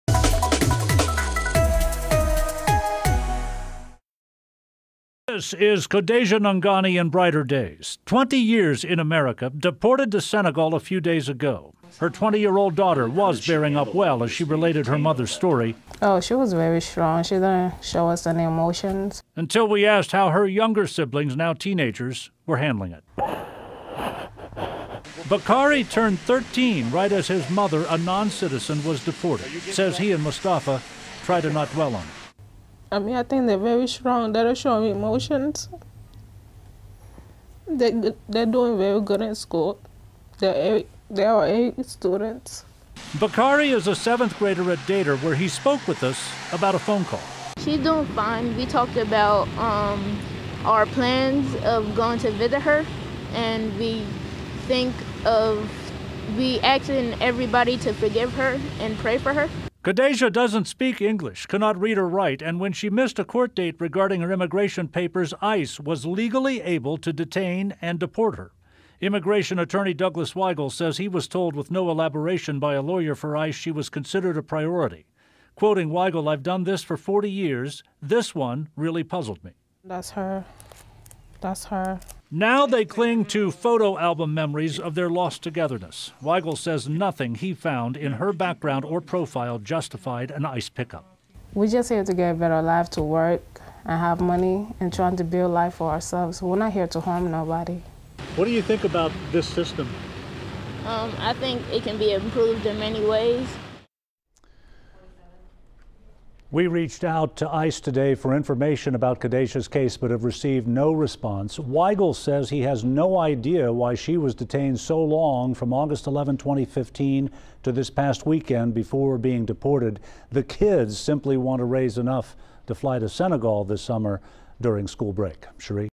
West side mother of 3 deported to Africa after 20 years in US
jl try 2